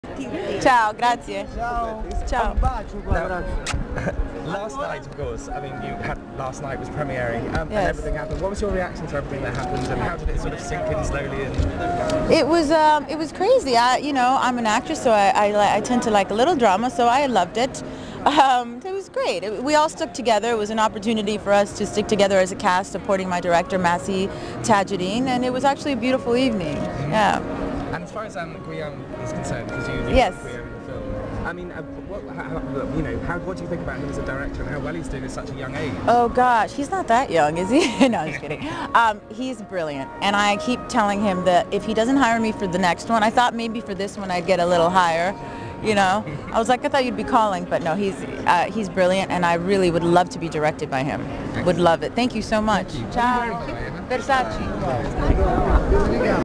Eva Mendes Rome Film Festival
Eva Mendes about movie Last Night and Guillaume Canet as director( Les petits mouchoirs )